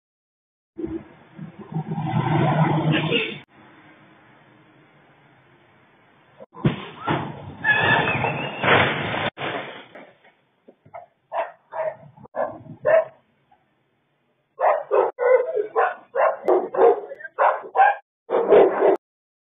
Una patrulla de la Guardia Nacional, perdió el control de la unidad en la autopista México-Puebla
Así quedó una patrulla de la Guardia Nacional, los ocupantes perdieron el control de la unidad en la autopista México-Puebla, a la altura de San Martín Texmelucan. Terminó su camino cuando se estampó contra un árbol